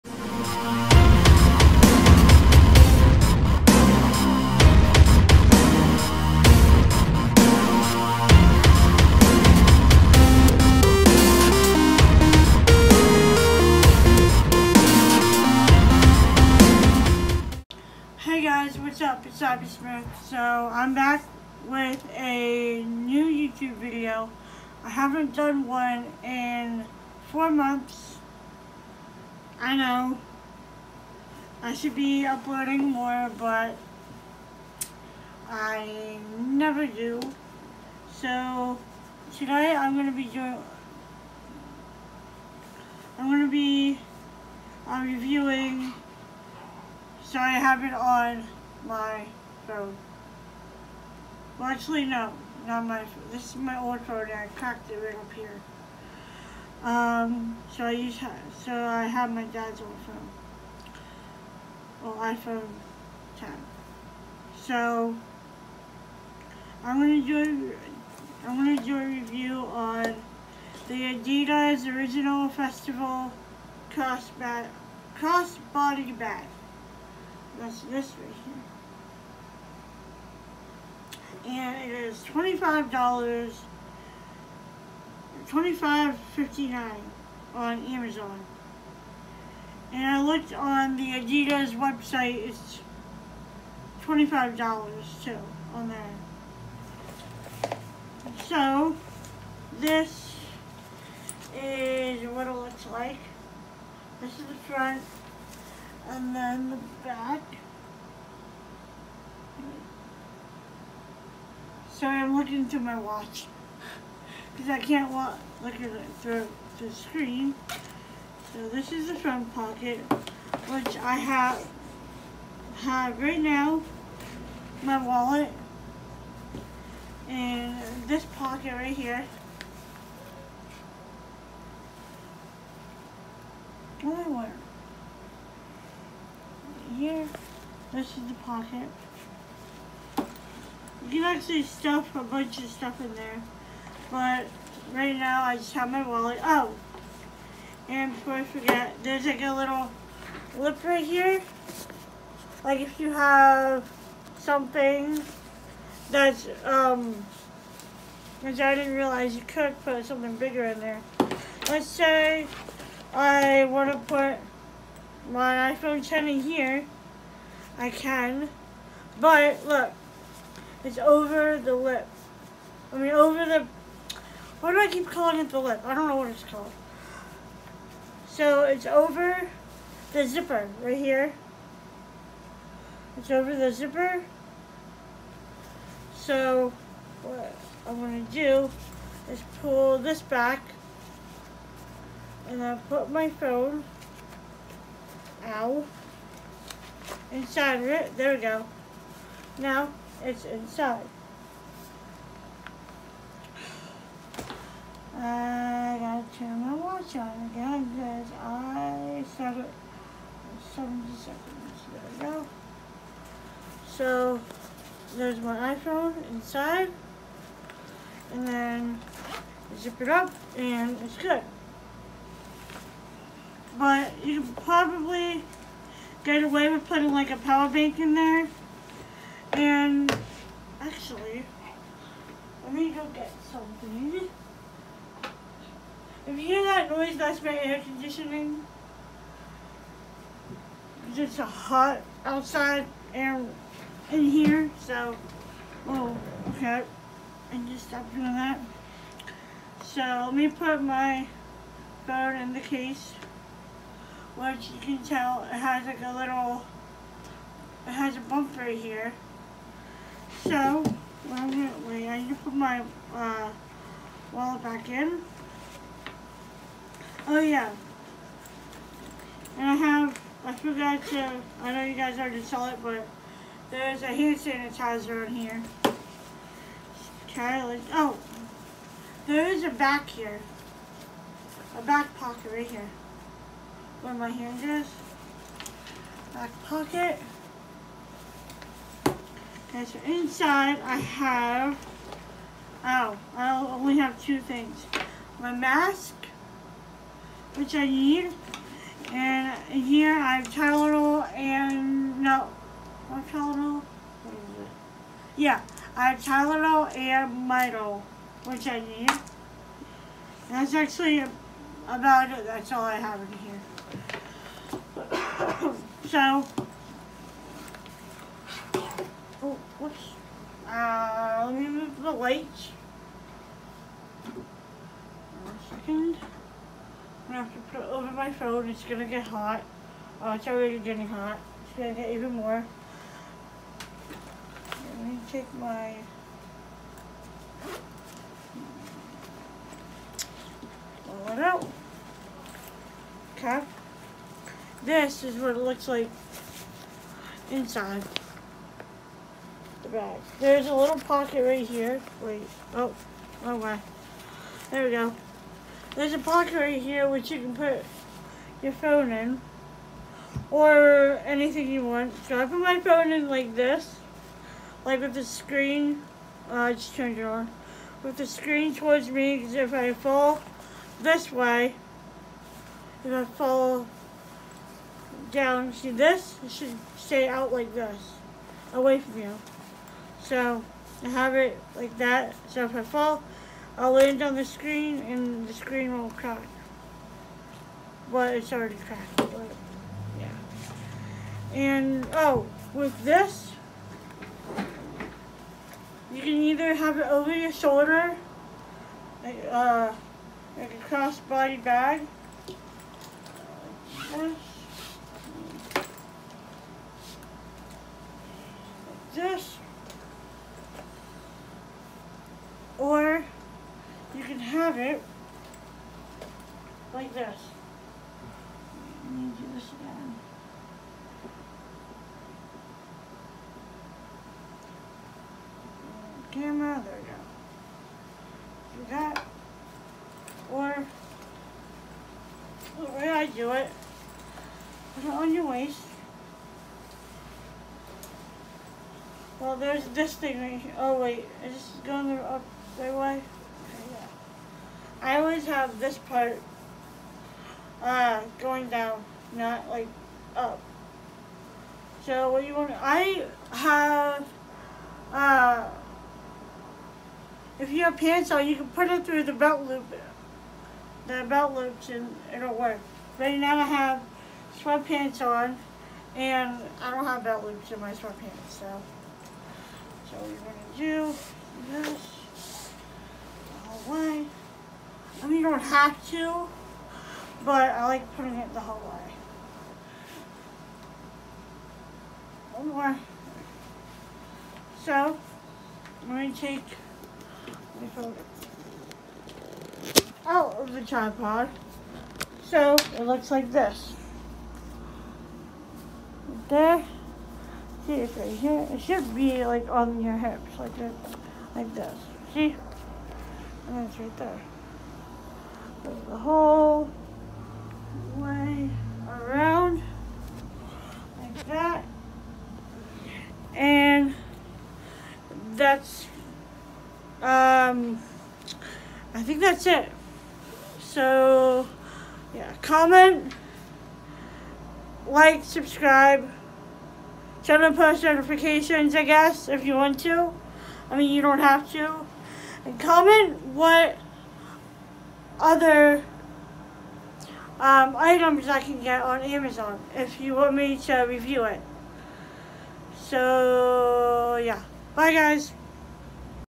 my review on the adidas Originals Festival Crossbody Bag
i got it on amazon for $25.54

i love the  bag.